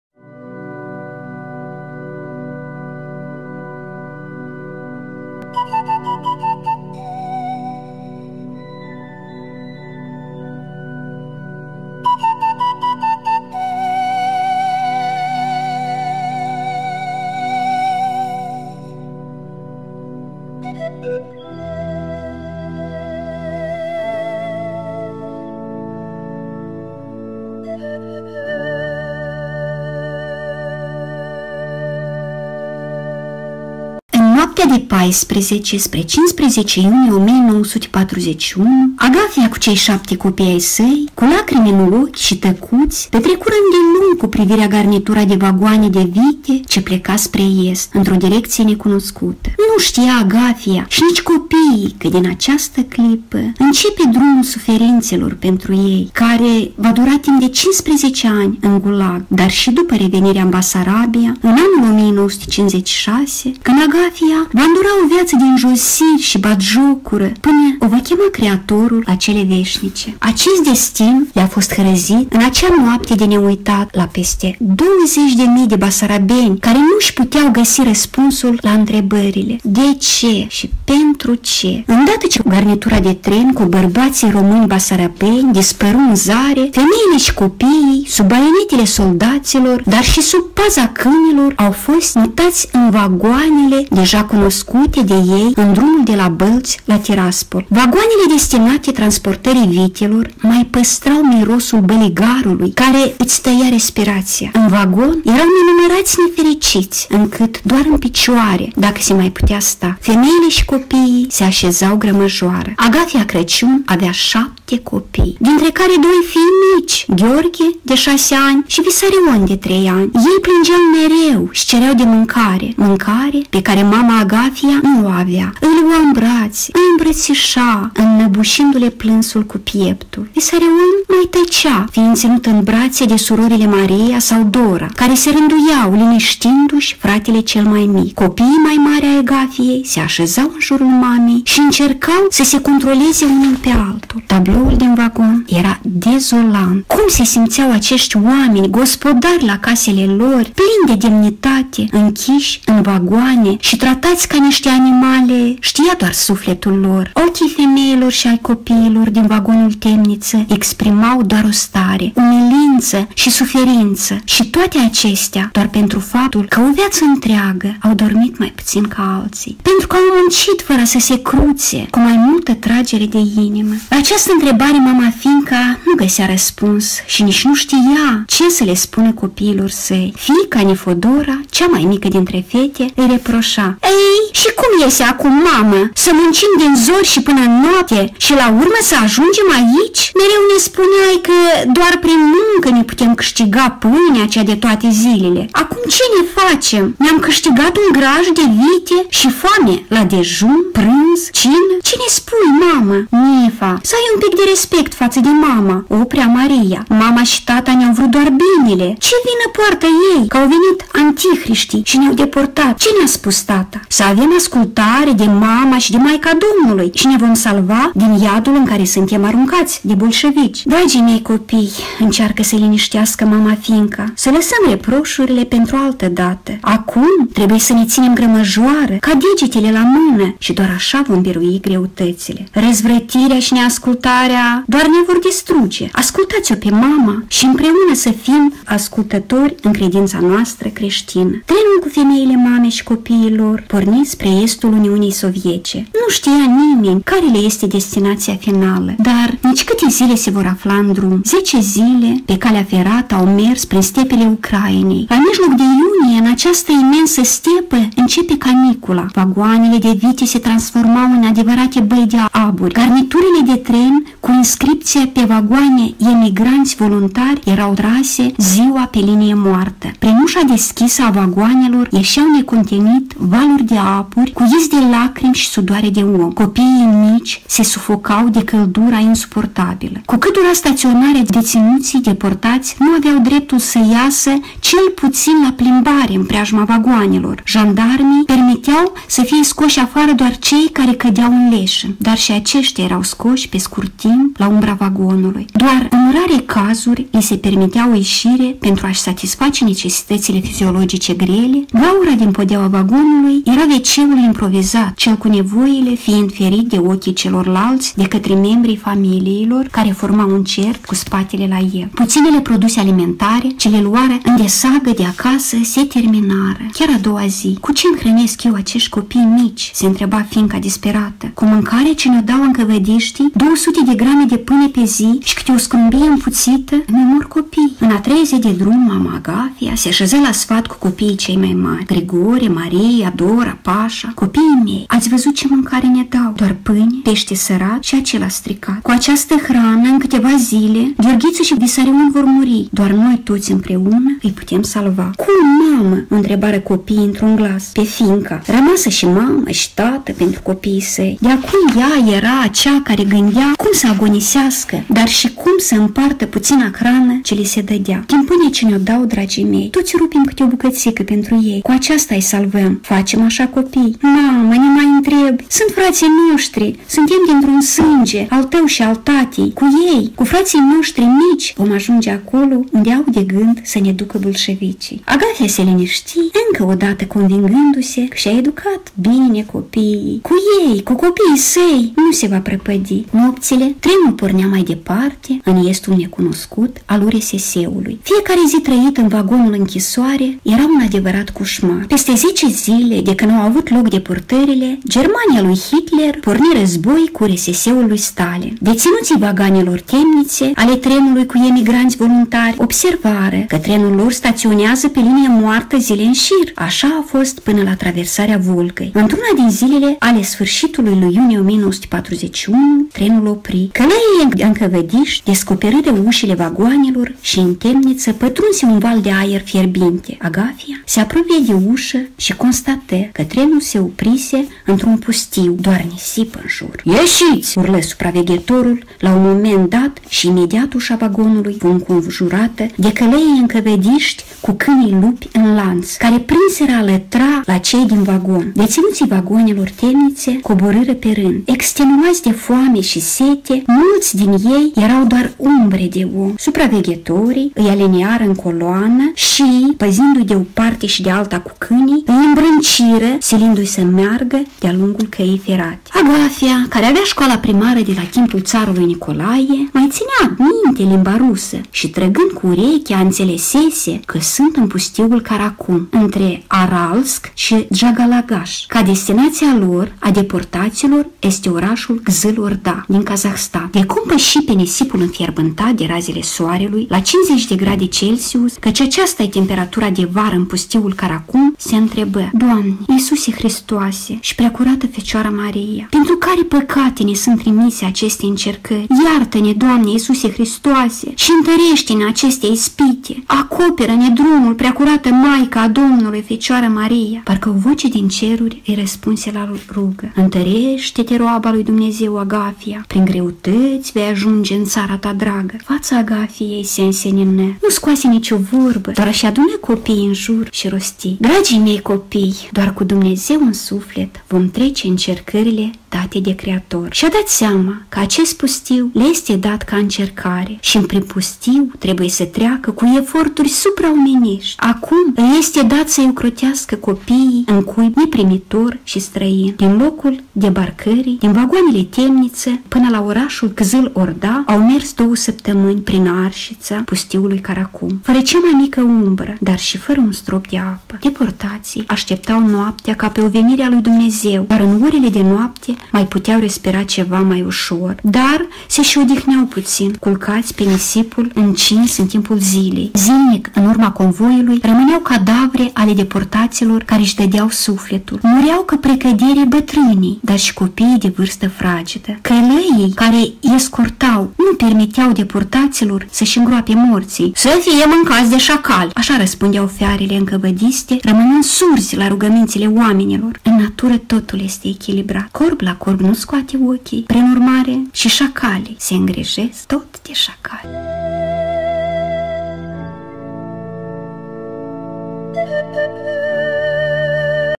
112. Vladimir Hotineanu „Cu dor de Basarabia”
Lilia Plinschi, pedagog din satul Mihalașa raionul Telenești, a lecturat din cartea lui Vladimir Hotineanu „Cu dor de Basarabia”, în cadrul Maratonului de Lectură Colectivă inițiat în Campania Raională de Lectură „Citim în familie” – Epizodul 112